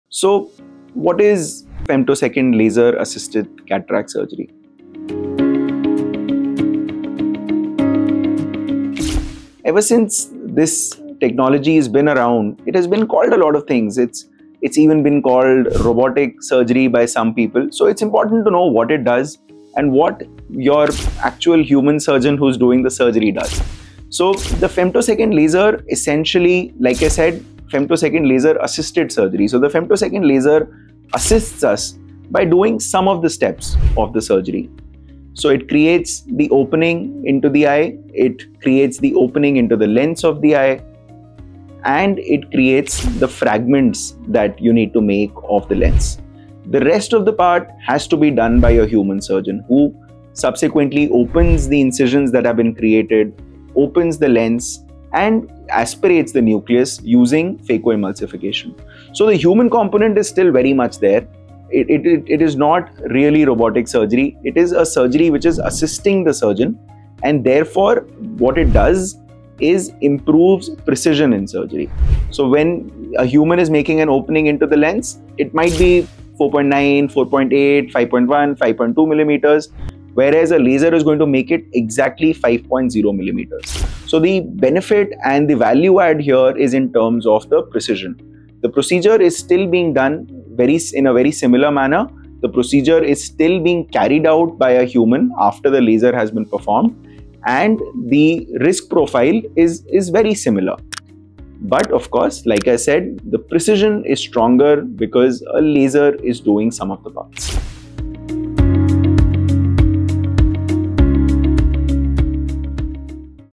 What is Femtosecond Laser Cataract Surgery?
Discover the truth about femtosecond laser cataract surgery in this myth-busting video! Learn how this advanced technology enhances precision by assisting your surgeon during critical steps of the procedure, such as creating precise openings and lens fragmentation. Despite common misconceptions, this isn’t robotic surgery—your skilled surgeon remains in control throughout the process, ensuring safe and effective results.

Whether you're exploring cataract surgery options or curious about the latest in eye care technology, this video provides clear, trustworthy insights to guide your decisions.

 🏣Location: 19, Siri Fort Road, Near Gargi College, New Delhi – 110049